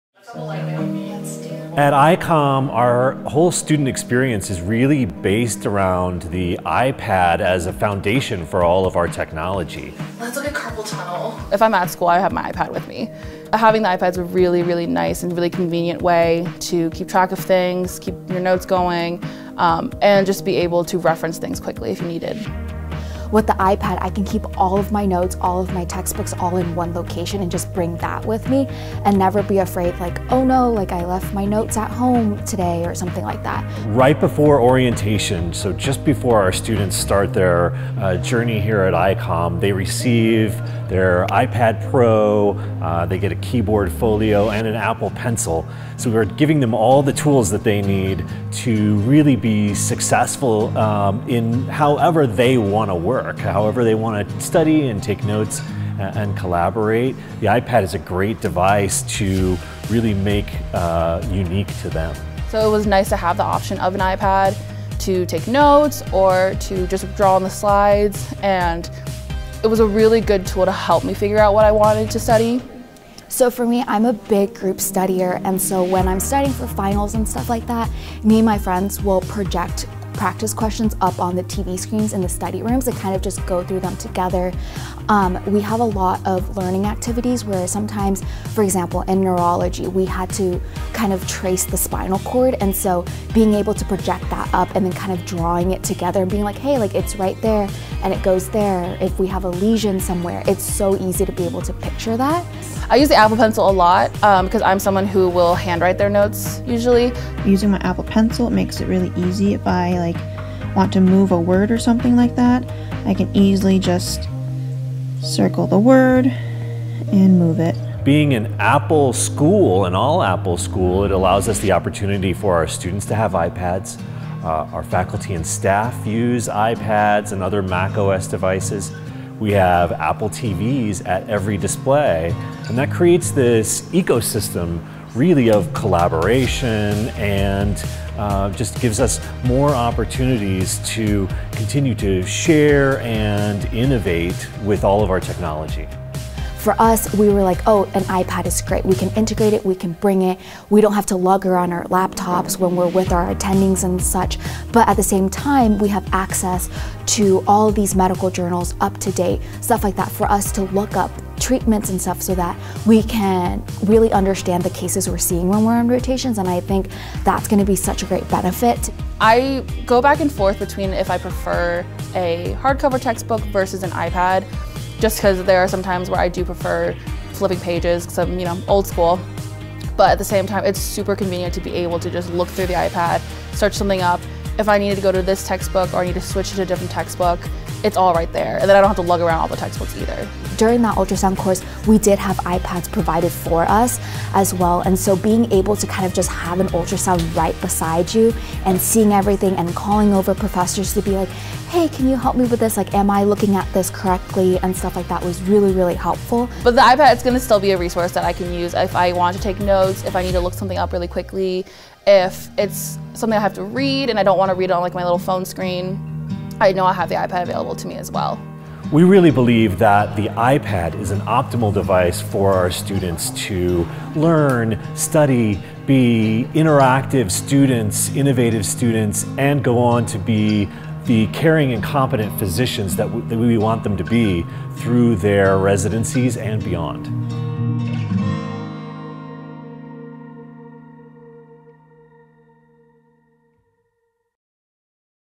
iPad at ICOM
At the Idaho College of Osteopathic Medicine, our technology infrastructure is built around iPad. Every ICOM student is given an iPad, Apple Pencil and Keyboard Folio. Whether you are taking notes during lectures, collaborating with other students, prepping for board exams, or checking references during rotations - iPad is a powerful, portable, and flexible device that empowers you to work and study on your own terms.